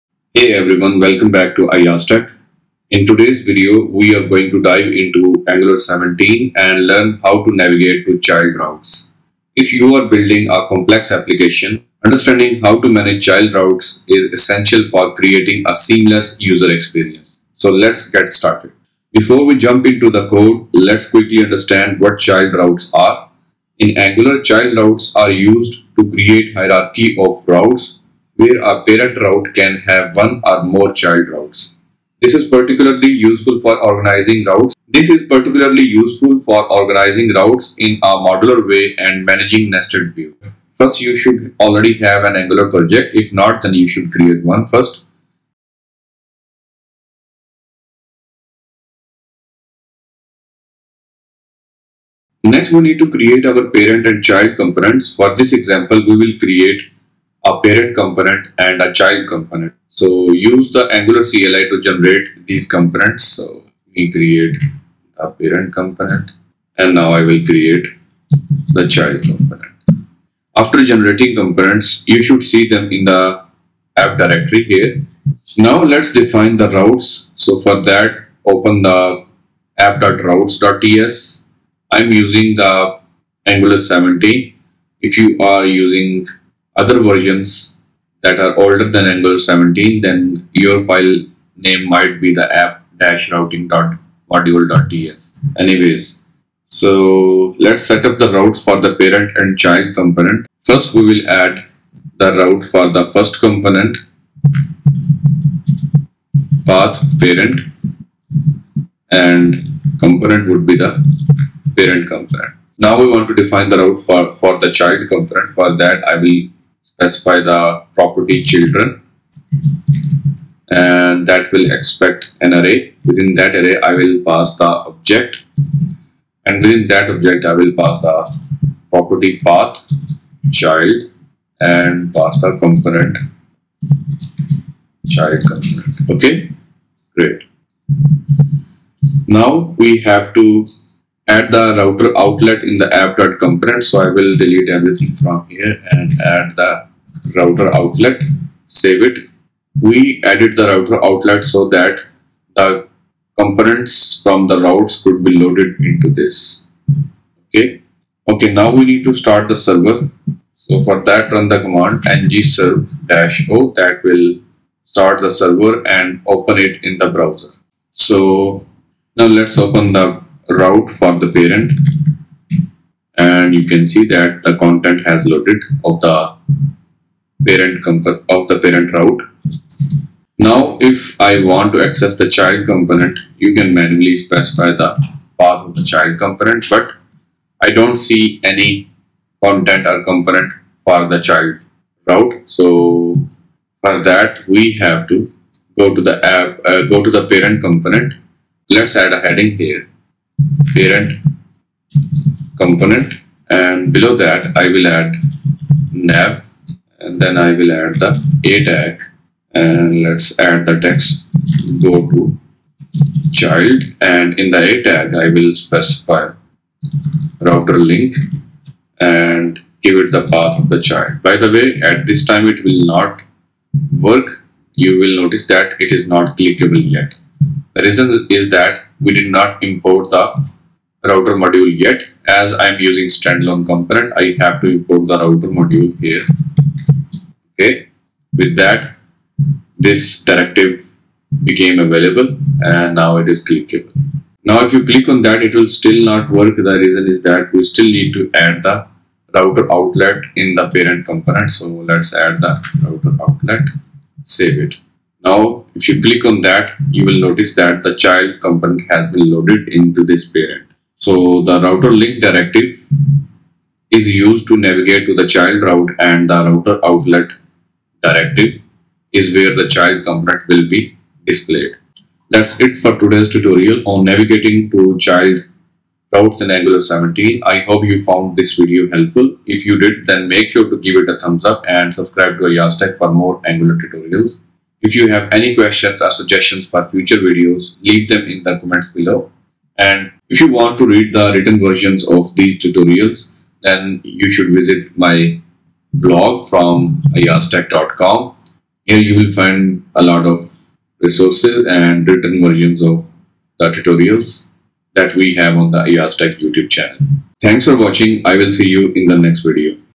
How to navigate to Child Route in Angular 17?
blog for articles of tutorials on AyyazTech channel.

I dived into Angular 17 in this video tutorial, focusing on navigating to child routes. Understanding child routes is crucial for building complex applications and providing users with a seamless experience.

Chapters:
00:00 Introduction to Child Routes in Angular 17
00:51 Creating Parent and Child Components
01:24 Defining Routes in app.routes.ts file
02:22 Adding Router Outlet in app.component
03:00 Adding Navigation to Child Component
04:05 Testing and Verifying Child Route Navigation
05:08 Conclusion and Closing Remarks